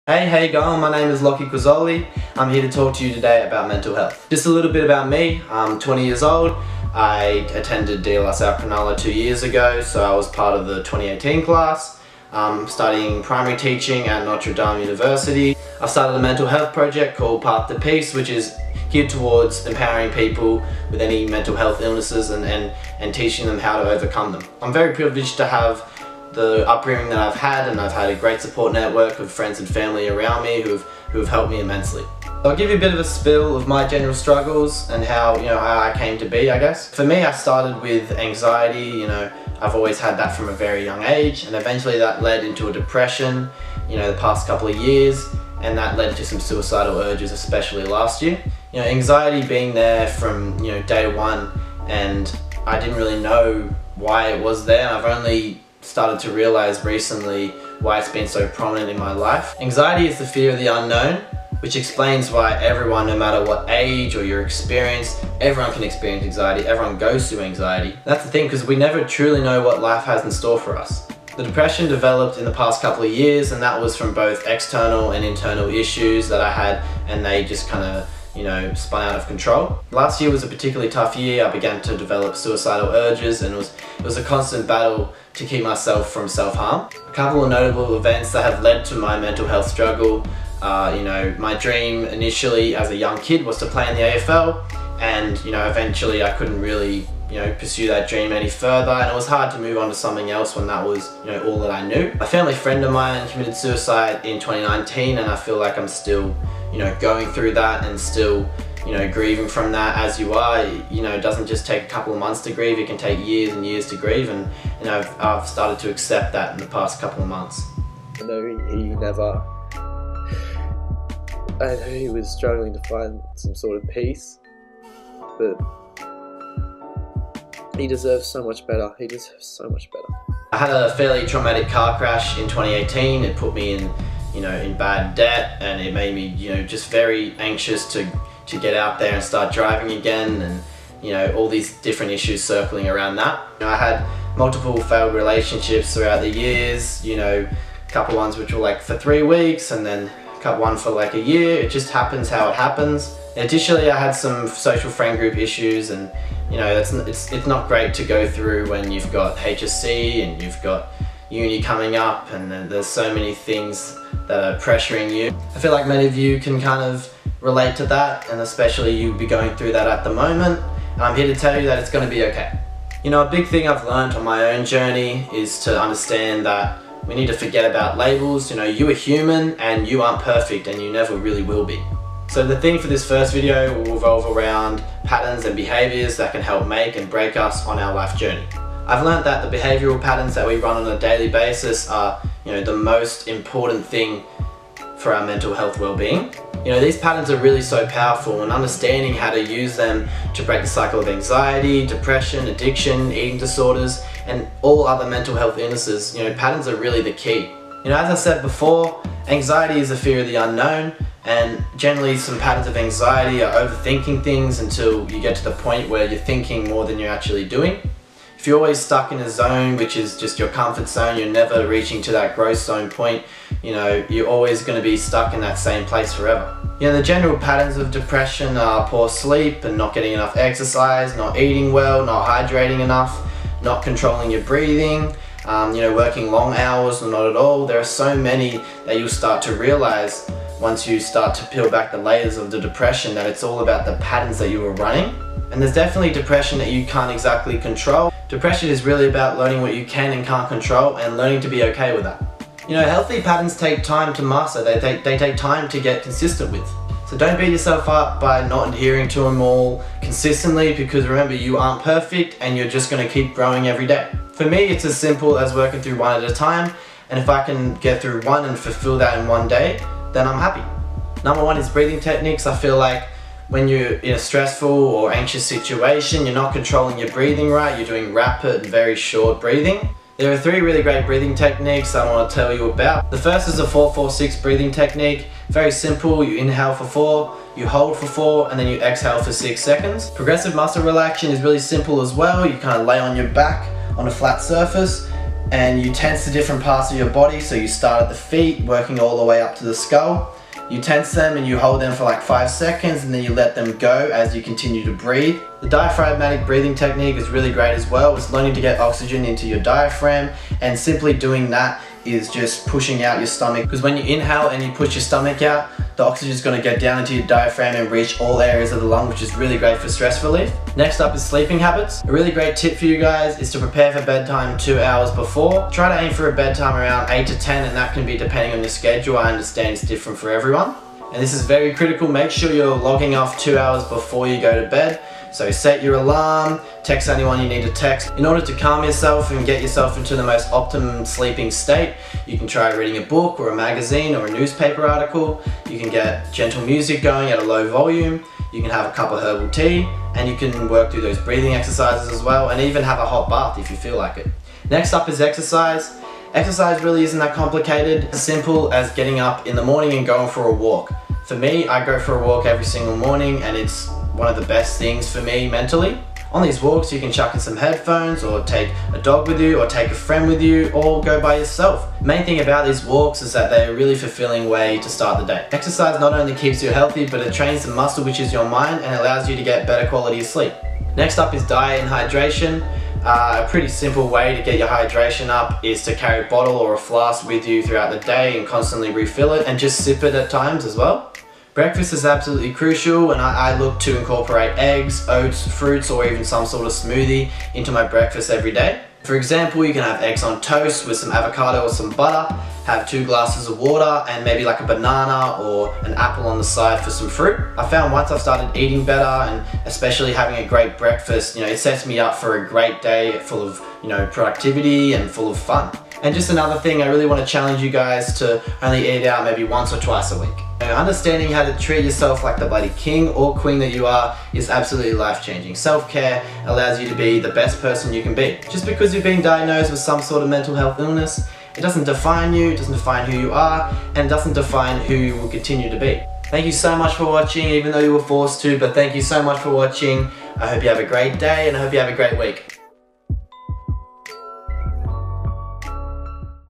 School Talk 1: Patterns
This is the first edition of videos I've prepared for students at De La Salle Catholic College Cronulla. Being my old school, I am so bloody grateful for this opportunity.

This video focuses on an overview of me and my struggles, and how behavioural patterns are so powerful towards our mental stability.